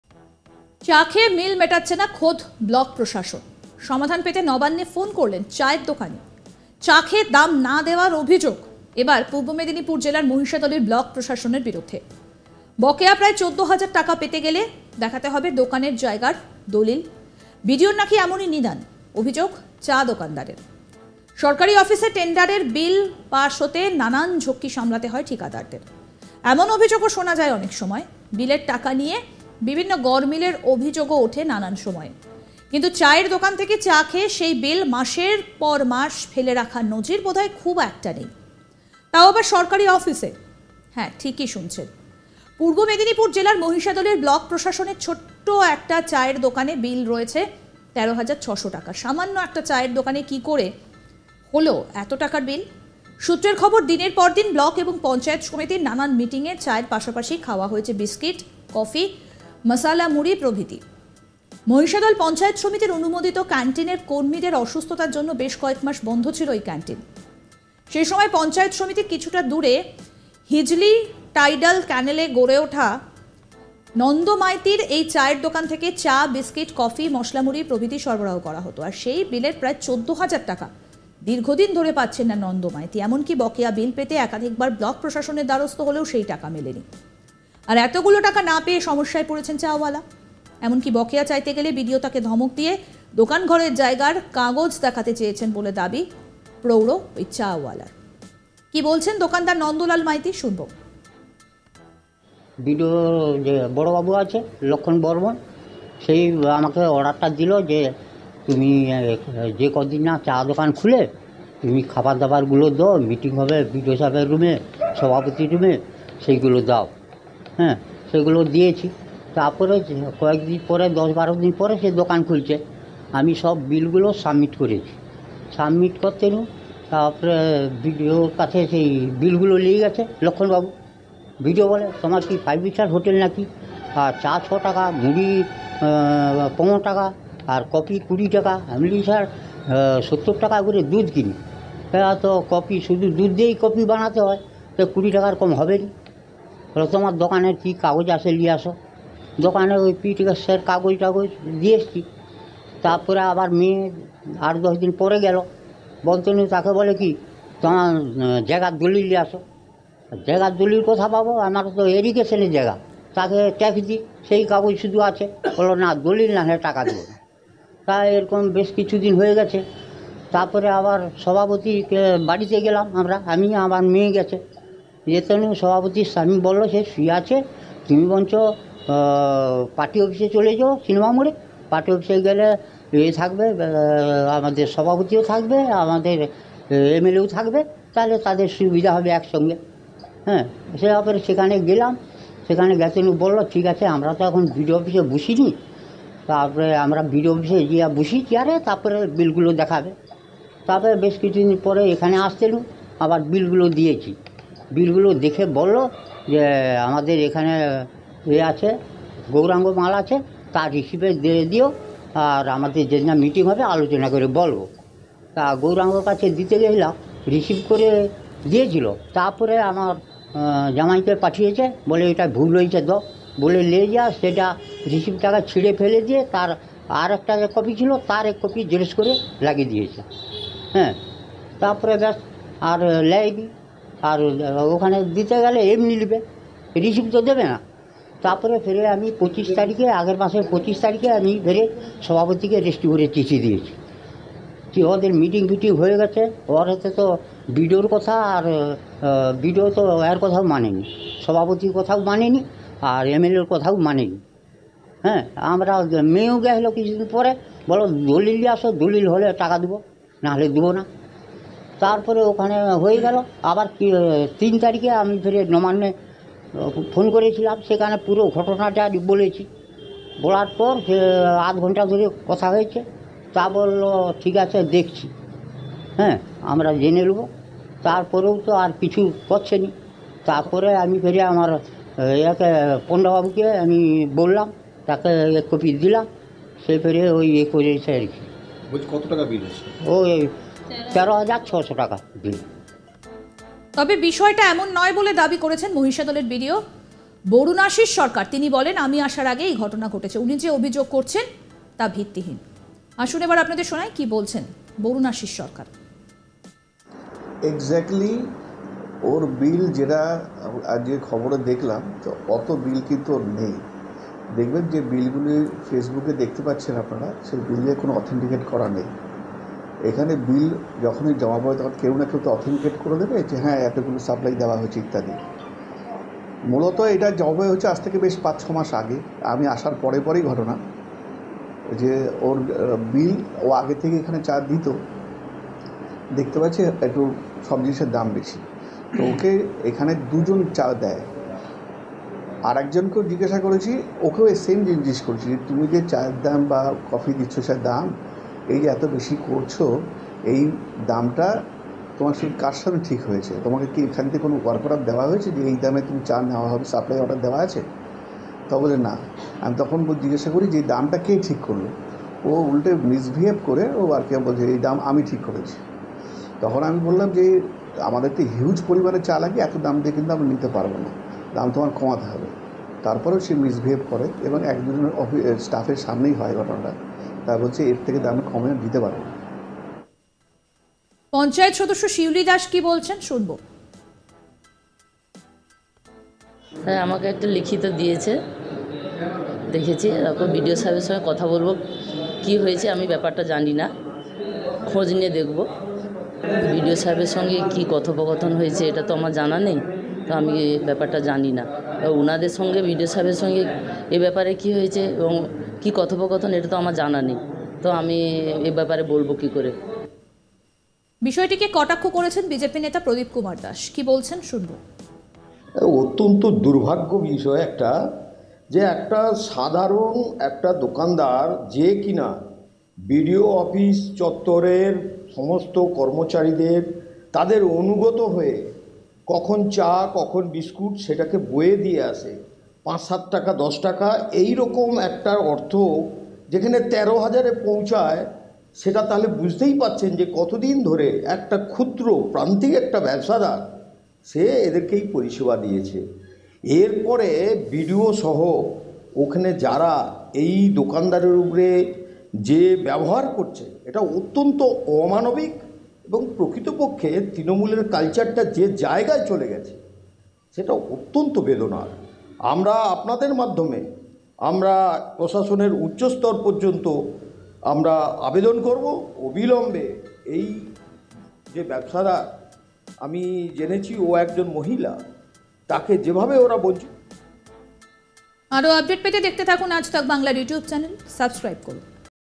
দিদিকে বলো নম্বরে চা-ওয়ালার ফোন, '14000 টাকার BDO-র খাওয়া চায়ের বিল বাকি' |East Midnapore|Tea Seller
চা খেয়ে বিল মেটাচ্ছেনা খোদ ব্লক প্রশাসন, সমাধান পেতে নবান্নে ফোন করলেন চা দোকানি। চা খেয়ে দাম না দেওয়ার অভিযোগ এবার পূর্ব মেদিনীপুর জেলার মহিষাদলের ব্লক প্রশাসনের বিরুদ্ধে। বকেয়া প্রায় 14 হাজার টাকা পেতে গেলে দেখাতে হবে দোকানের জায়গার দলিল, বিডিওর নাকি এমন নিদান, অভিযোগ চা দোকানদারের। সরকারি অফিসে  টেন্ডারের বিল পাশ হতে নানান ঝক্কি সামলাতে হয় ঠিকাদারদের, এমন অভিযোগ শোনা যায় অনেক সময়। বিলের টাকা  নিয়ে বিভিন্ন গরমিলের অভিযোগও ওঠে নানান সময়ে। কিন্তু চা দোকান থেকে চা খেয়ে সেই বিল মাসের পর ফেলে রাখার নজির বোধহয় খুব একটা নেই, তাও আবার সরকারি অফিসে। হ্যা, ঠিকই শুনছেন। পূর্ব মেদিনীপুর জেলার মহিষাদলের ব্লক প্রশাসনের ছোট্ট একটি চা দোকানে বিল বাকি রয়েছে ১৩ হাজার ৬০০ টাকা। সামান্য একটা চা দোকানে  কি করে হলো এতো টাকার বিল।